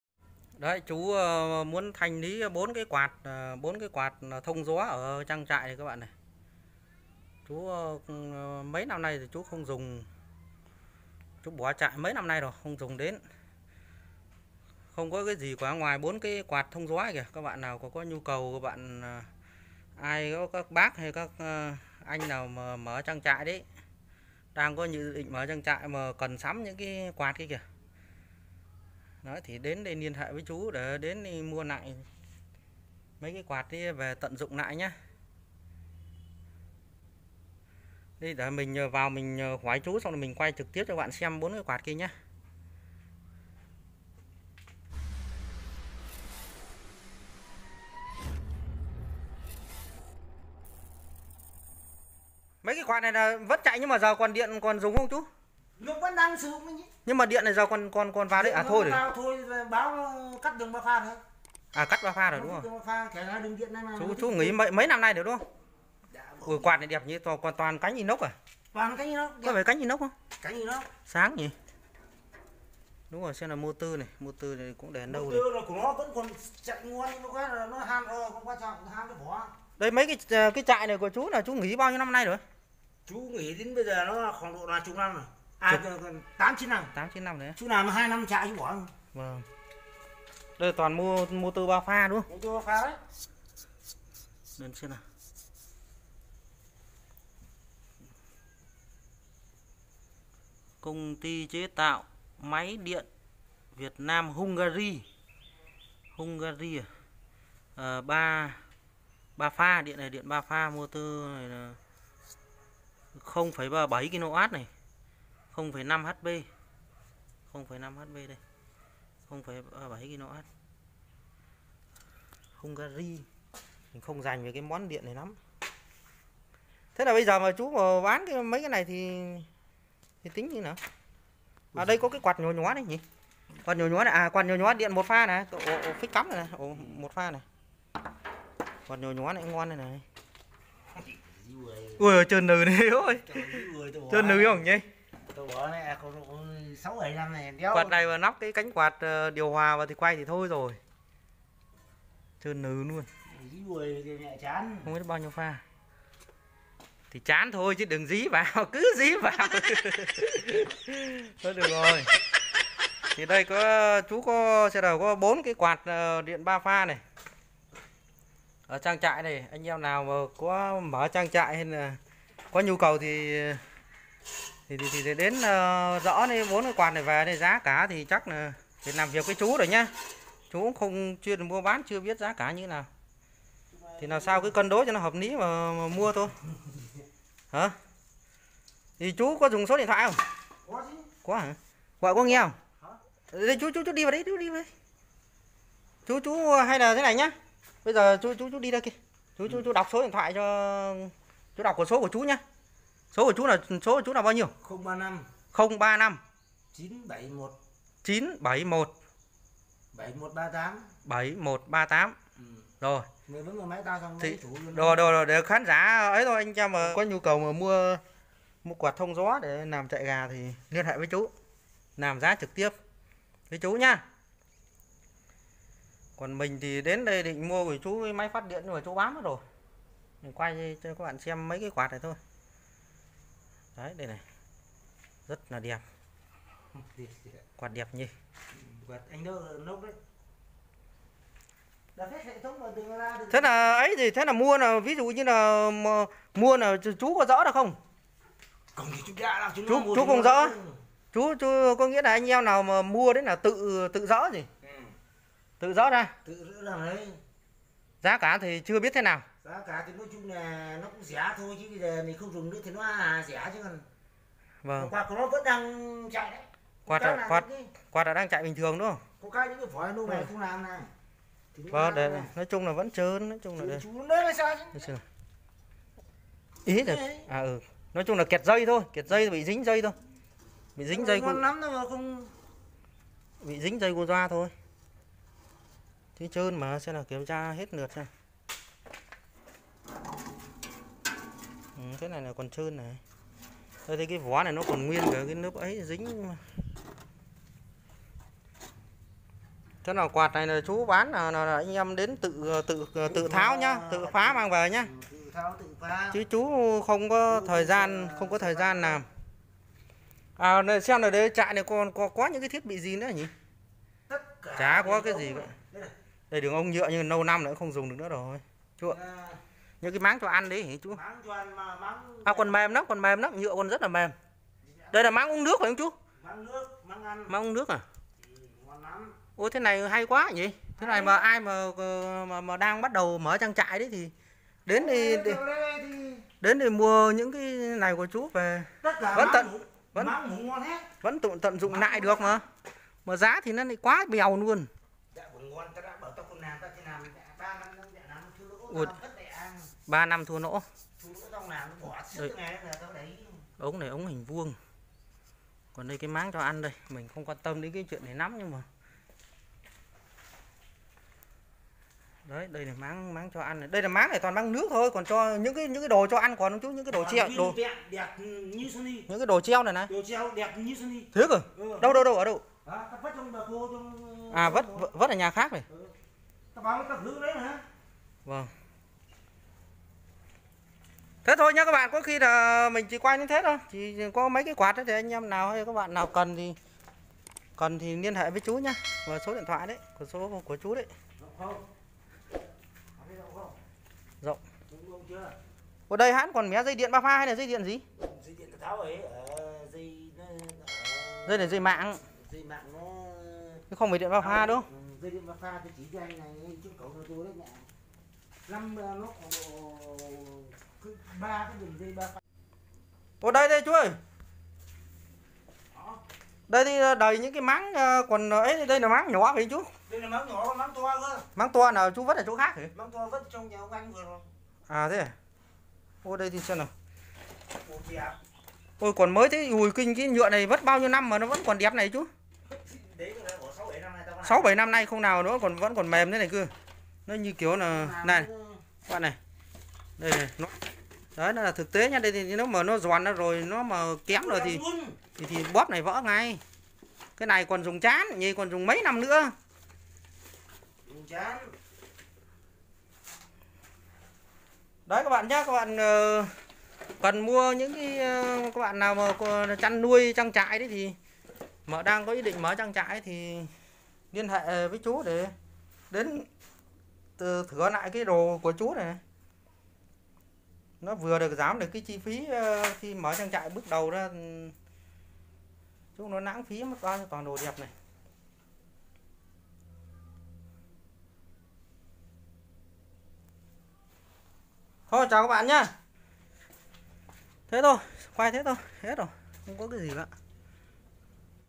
Thanh lý 4 quạt thông gió ở trại gà, số đt trong phần mô tả.
Chào mừng các bạn đã đến với kênh Lộc máy 💓 chúc các bạn xem video vui vẻ .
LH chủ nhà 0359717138